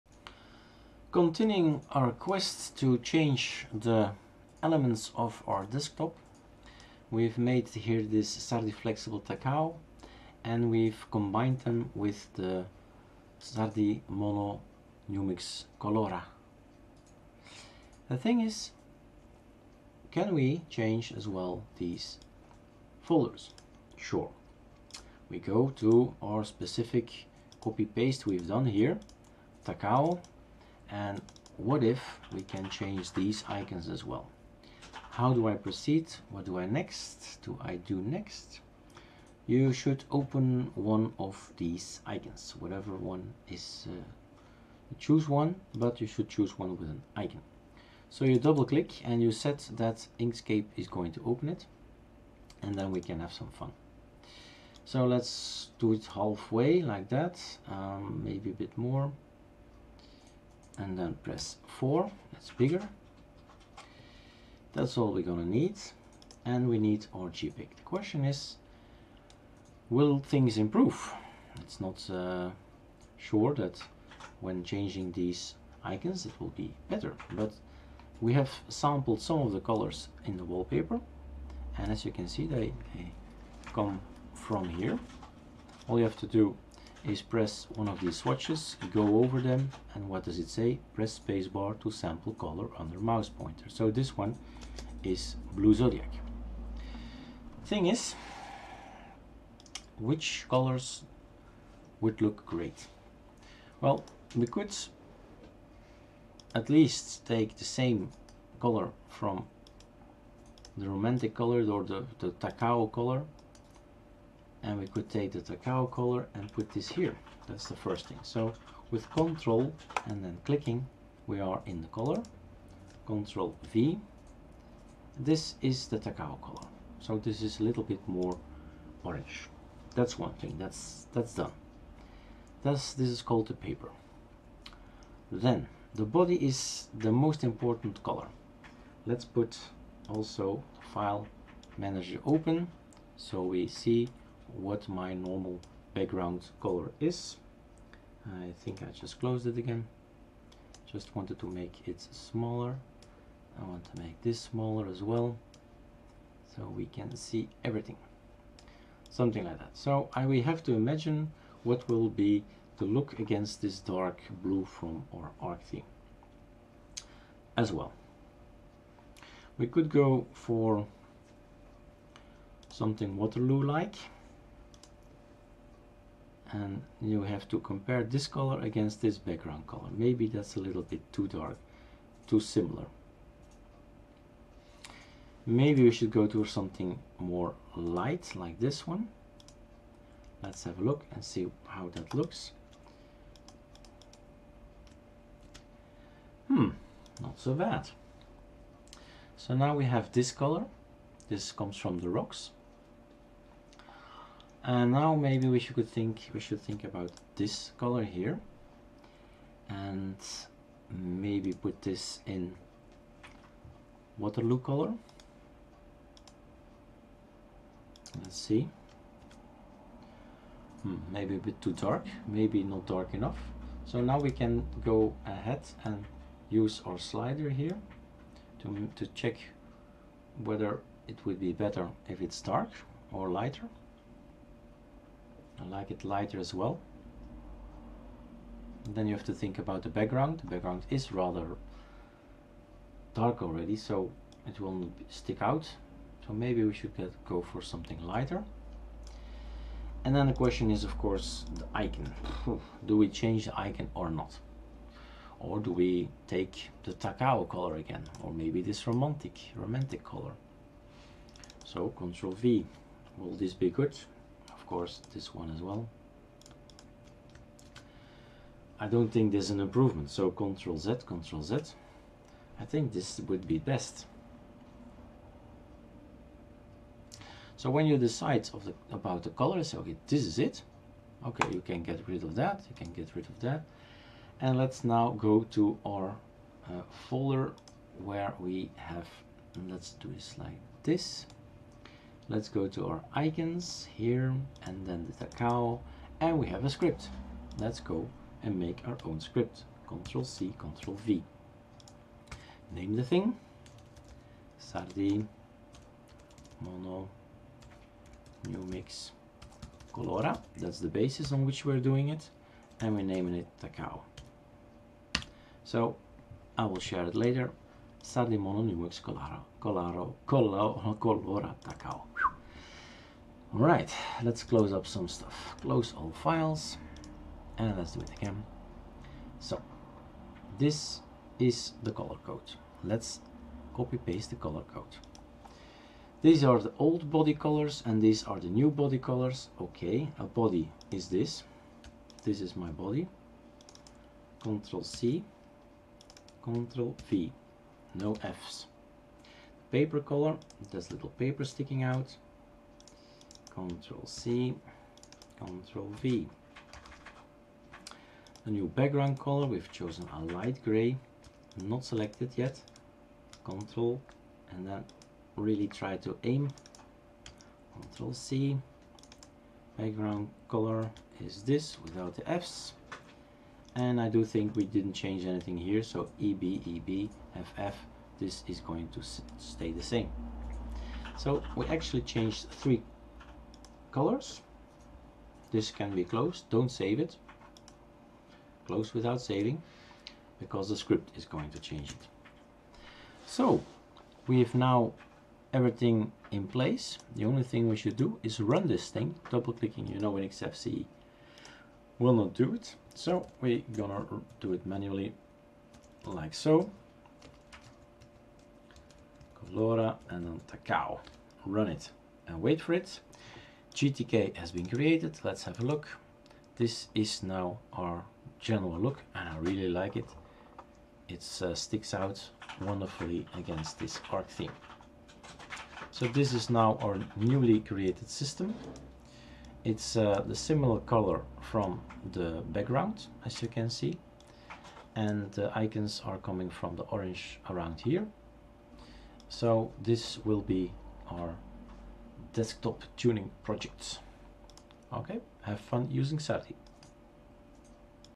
Complete desktop tuning Tacao 2/4 creating Sardi Mono Numix Colora Tacao
Working with the Sardi icons, Aureola Conky and Arc theme colora we will make a complete desktop tuning project.

Sardi icons objectives :
Sardi is distro independent,
Sardi is lean and clean,
Sardi is flexible,
Sardi is modular,
Sardi is diverse